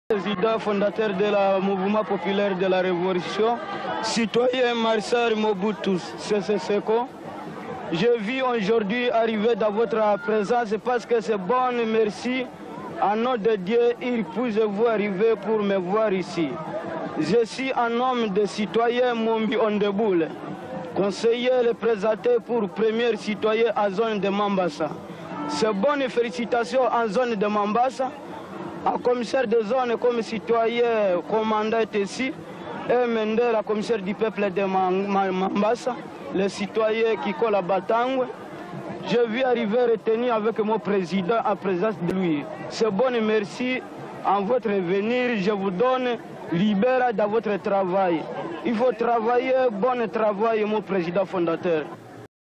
Discours du premier citoyen de Mambassa face à Mobutu
"Citoyen a Marechal MOBUTU SESE SEKO.

Je vie aujourd’hui arrivé dans votre en présence parce que ce bonne merci, à mon Dieu ici je veux arrivé pour voir ici.

Je suis à nomme du Citoyen MUNDI ONDEMBULE conseiller représenter pour premier Citoyen à Zone de Mambassa.

Ce bonne félicitation à Zone de Mambassa, à Commissaire de Zone comme Citoyen Commandant est ici un Mendera Commissaire du Peuple de Mambassa, le Citoyen KIKOOLE BATANGUE.

Je vi arrivé retenir mon Président à présence de lui, ce bonne merci à votre devenir je vous donne libera dans votre travail.